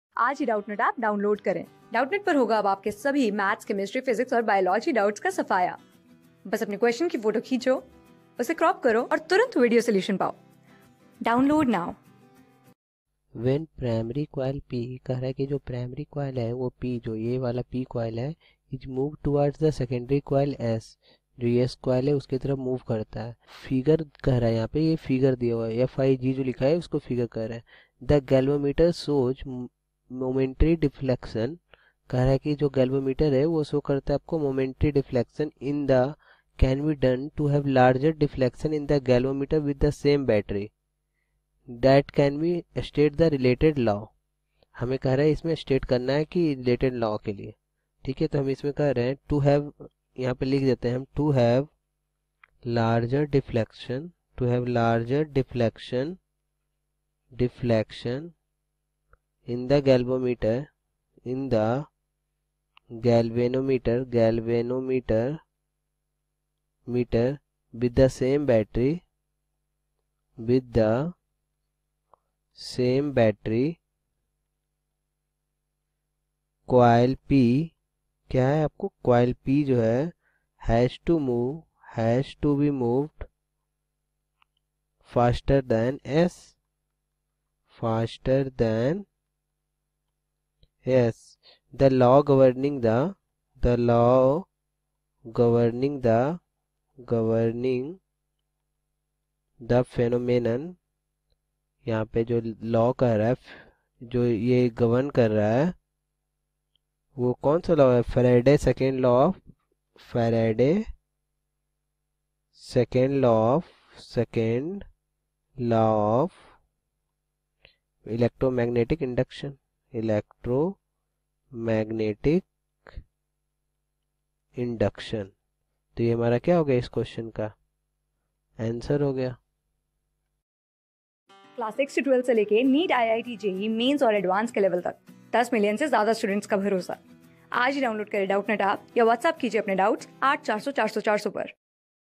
When primary coil P is moved towards secondary coil S, Fig. the galvanometer shows memetary defl...
When primary coil P is moved towards secondary coil S, Fig. the galvanometer shows memetary deflection in the can be done to have larger deflection in the galvanometer with same battery ? State the related law ?
Class: 12
Subject: PHYSICS
Chapter: ELECTROMAGNETIC INDUCTION & ALTERNATING CURRENT
Board:CBSE

👉 Have Any Query? Ask Us.